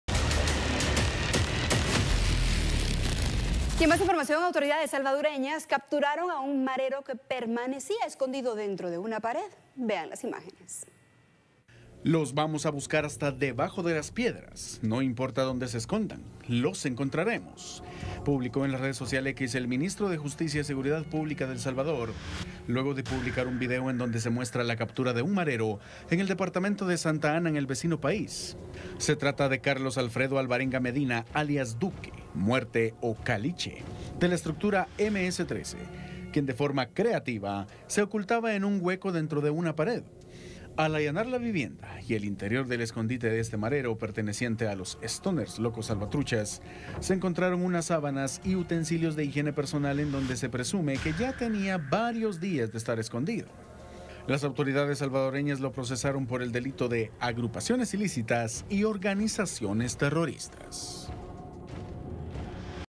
Capturan a peligroso pandillero que se escondía en un hueco
Un hombre que es un pandillero de la MS-13 se escondió dentro del hueco en una pared, pero eso no fue impedimento para que sea capturado por la policía de El Salvador.